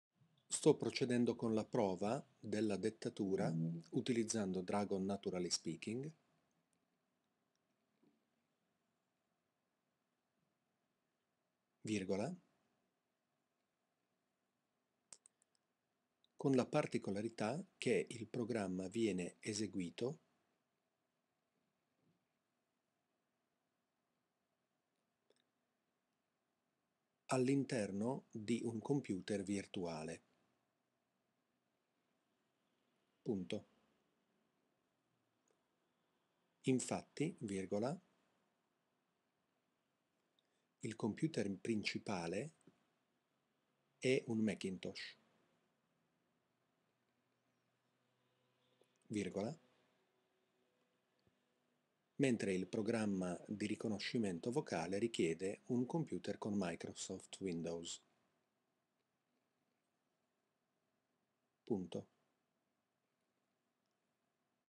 Dictating in a Macintosh using Dragon NaturallySpeaking
Dragon NaturallySpeaking is an excellent speech recognition program, with an outstanding accuracy right out of the box. It requires a Windows machine however, so it is good that it works with VMware Fusion which emulates Windows on the Macintosh. Here I am using it in Italian on my Mac. Obviously it works the same in English too!